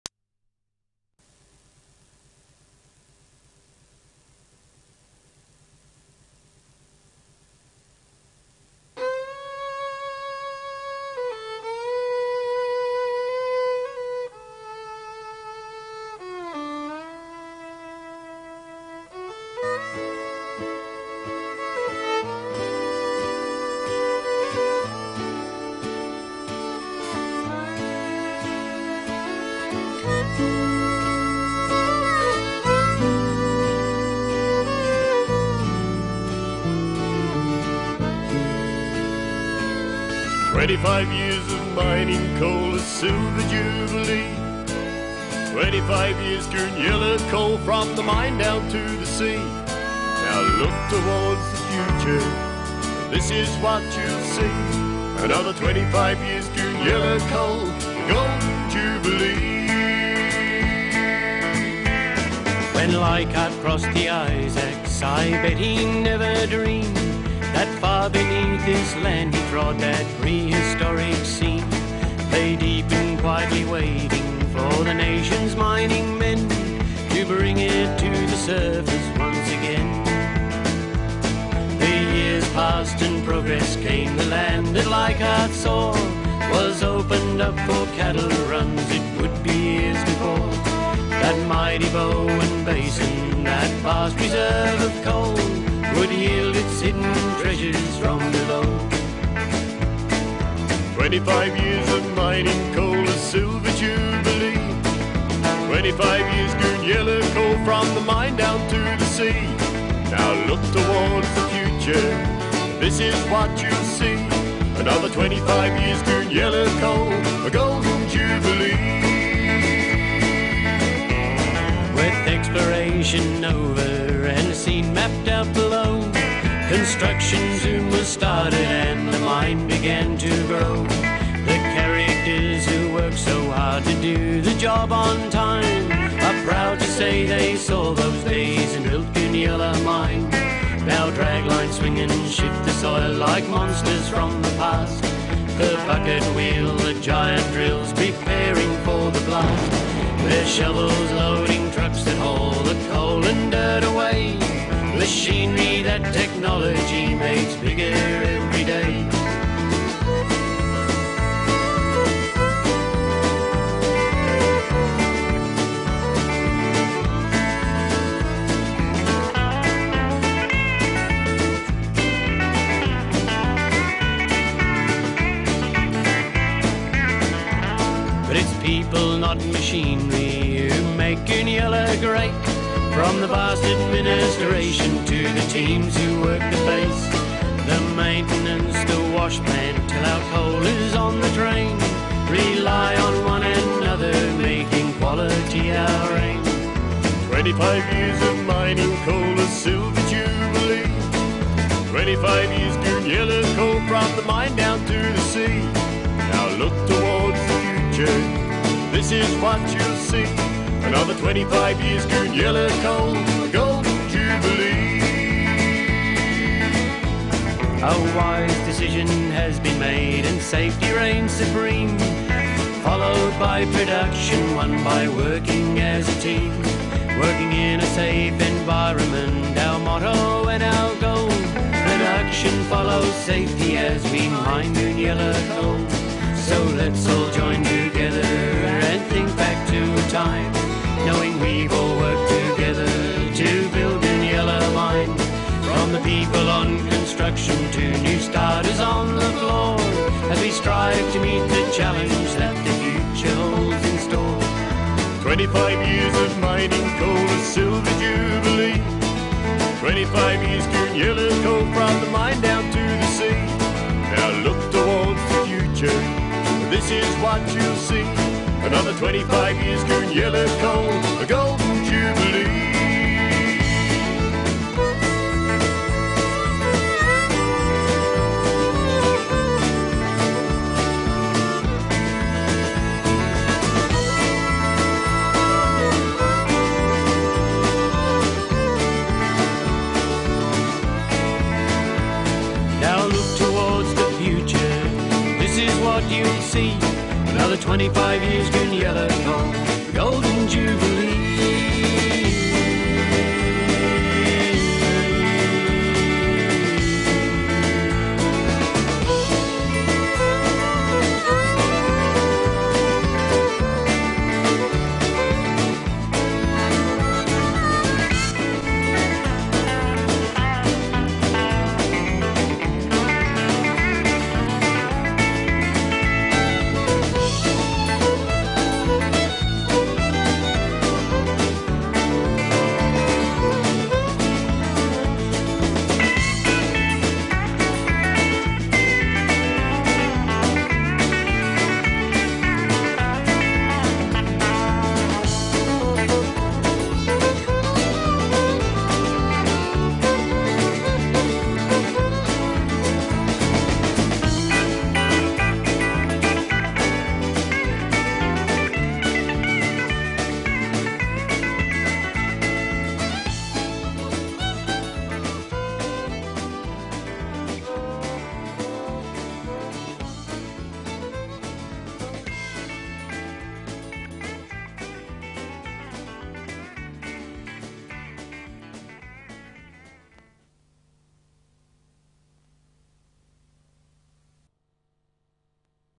BHP / BMA: Goonyella Mine 25 Year Silver Jubilee Video
Performed by Ross Burgum and Paul Sharp.